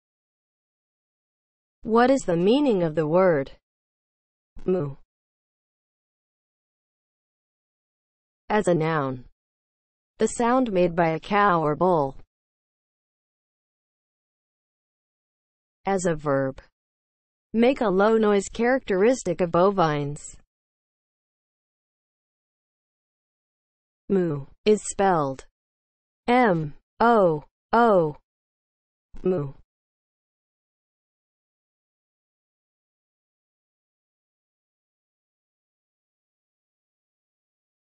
What is the meaning of the word MOO?
Definition and spelling of the word MOO.

(00:00) Intro
(00:07) As a noun
(00:14) As a verb
(00:22) Spelling

As a noun (00:07):
• the sound made by a cow or bull

As a verb (00:14):
• make a low noise, characteristic of bovines